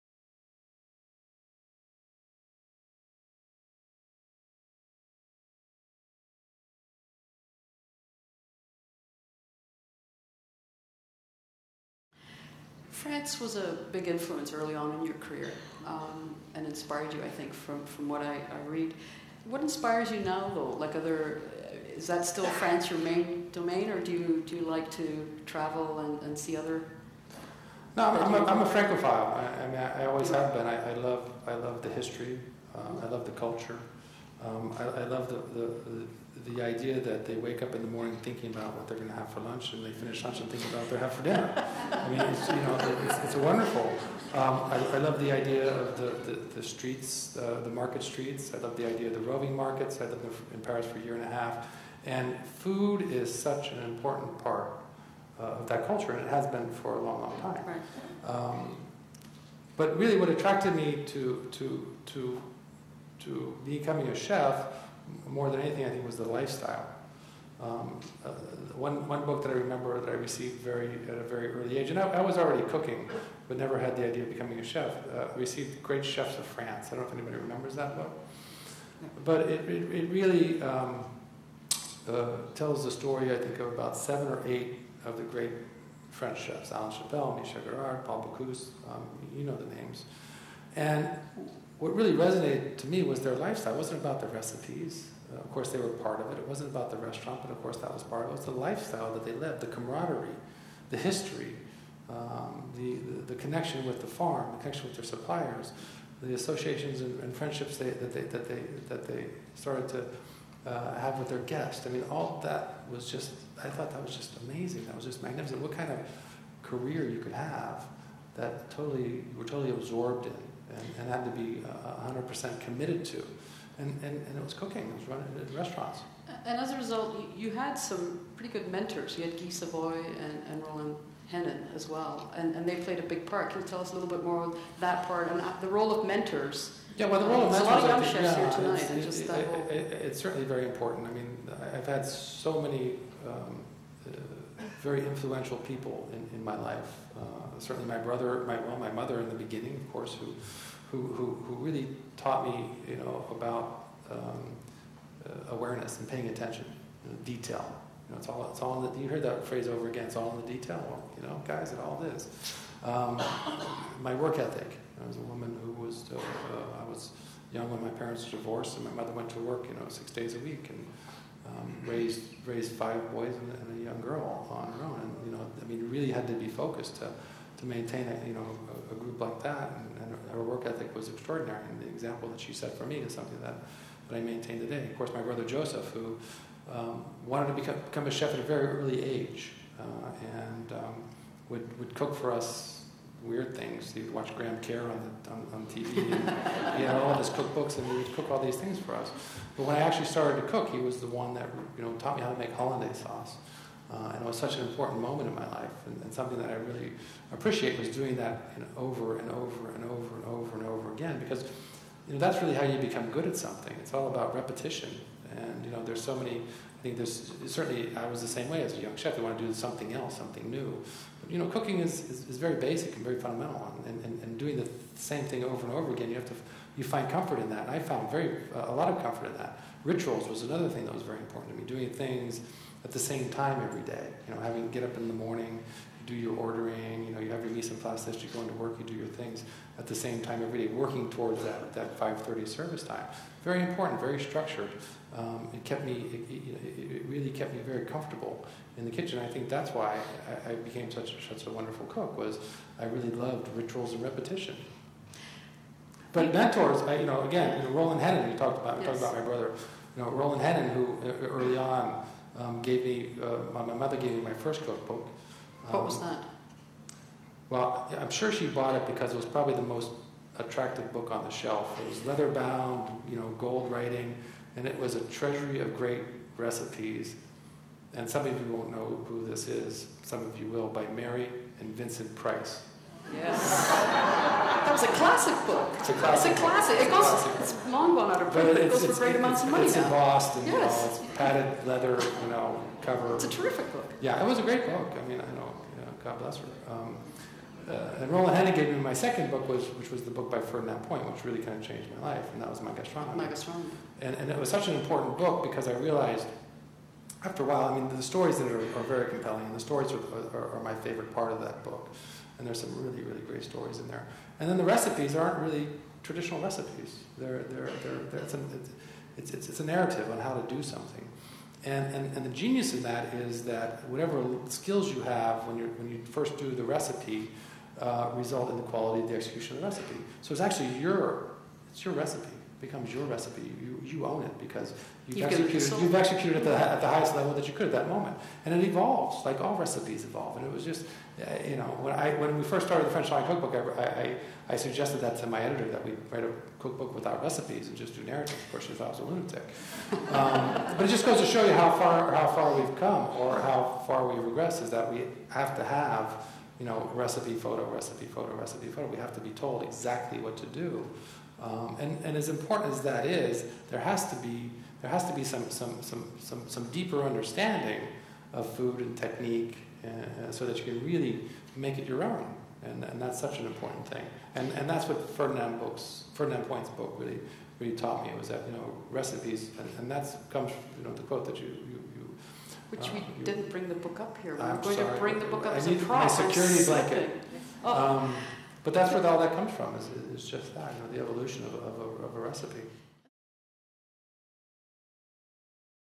Thomas Keller Interview Part 3 Cookbook Store
Alison Fryer of The Cookbook Store in Toronto, Canada interviews chef Thomas Keller.
Continued in Part 4.